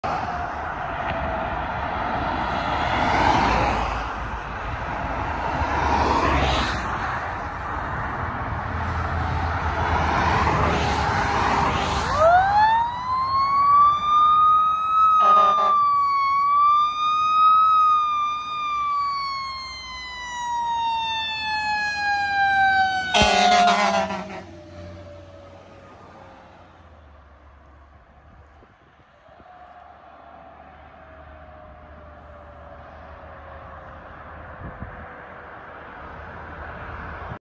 Springfield Fire Company Squad 44 and CKHS EMS Medic 103 Responding
Springfield Fire Company Squad 44 and CKHS EMS Medic 103 Responding to an accident on 476